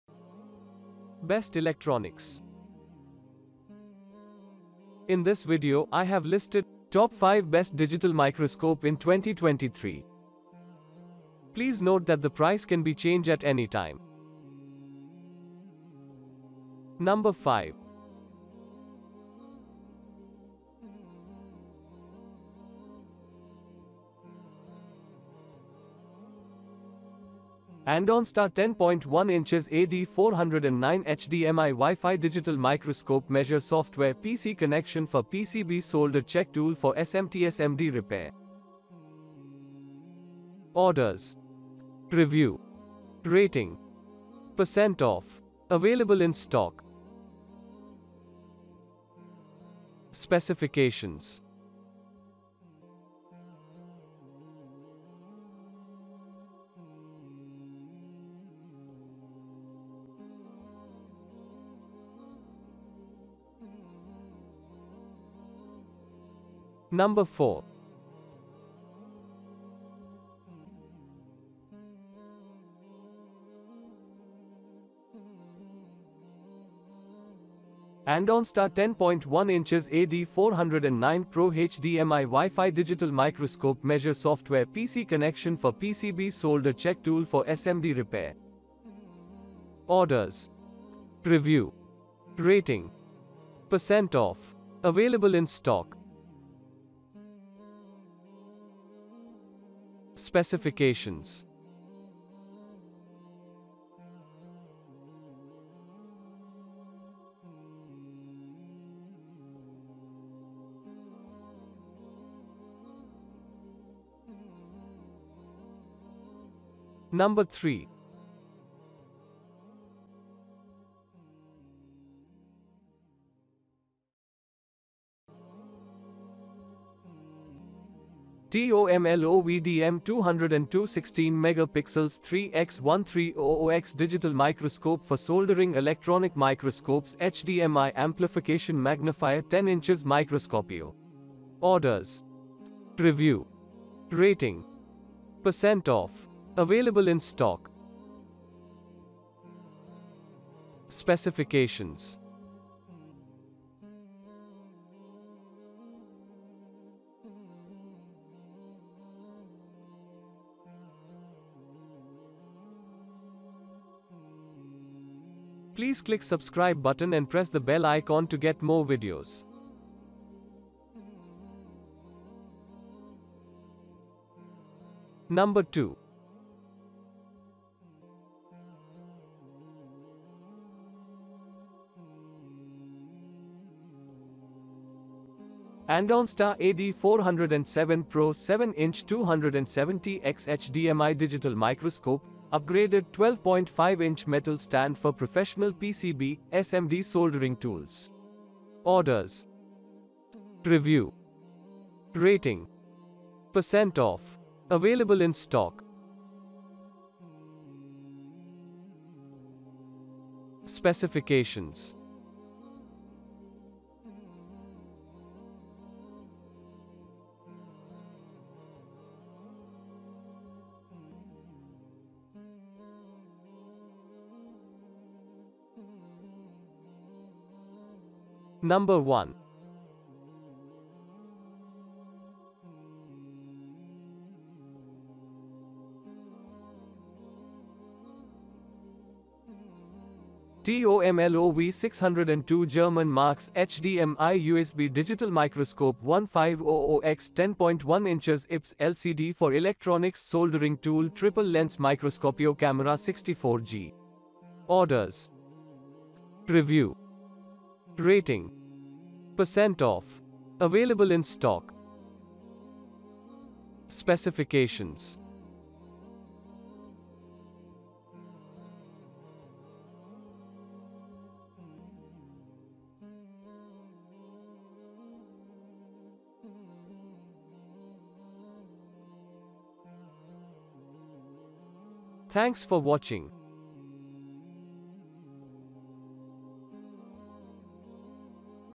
Top 5 Best Andonstar Digital Microscope 2023
5. Andonstar 10.1'' AD409 HDMI WIFI Digital Microscope Measure Software PC Connection for PCB Solder Check Tool

4. Andonstar 10.1'' AD409 Pro HDMI WIFI Digital Microscope Measure Software PC Connection for PCB Solder Check Tool

3. TOMLOV DM202 16MP 3X-1300X Digital Microscope for Soldering Electronic Microscopes HDMI Amplification Magnifier

2. Andonstar AD407 Pro 7 Inch 270X HDMI Digital Microscope,Upgraded 12.5 inch Metal Stand for Professional PCB

1. TOMLOV DM602 HDMI USB Digital Microscope 1500X 10.1" IPS LCD for Electronics Soldering Tool Triple Lens Microscopio Camera